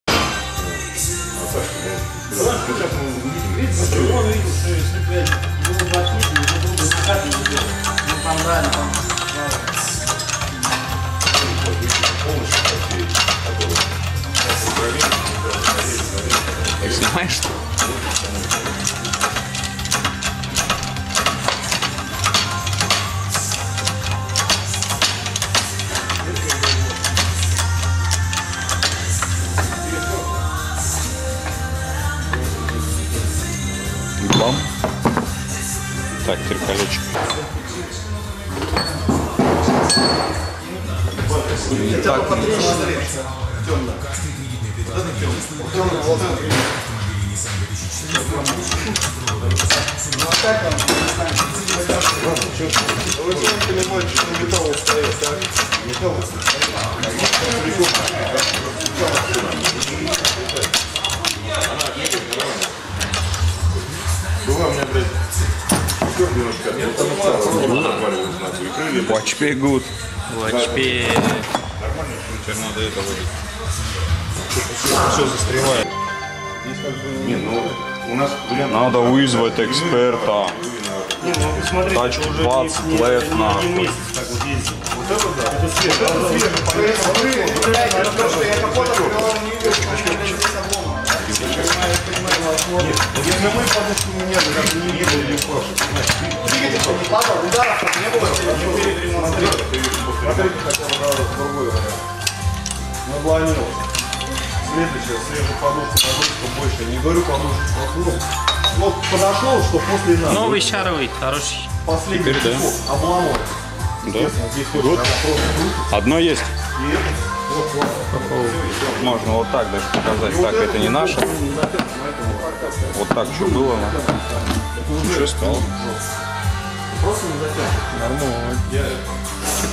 Замена шаровой опоры BMW e46
Многие спорят, что перепрессовывать шаровые - горячка. На собственном опыте я убедился, что это совсем не так.
 Ради эксперимента была заказана одна самая дешевая шаровая опора

Номера шаровых опор.
BMW part # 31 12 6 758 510
Meyle part# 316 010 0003/HD
Lemforder part# 10536 01